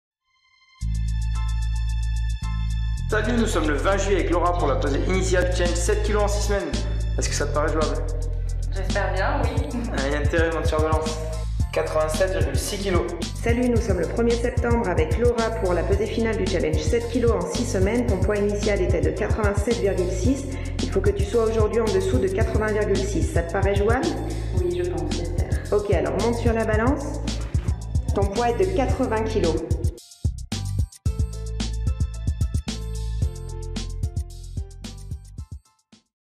Transformation physique rapide Lyon
Laura à perdus 7,600 kgs en 6 semaines à la Training Academy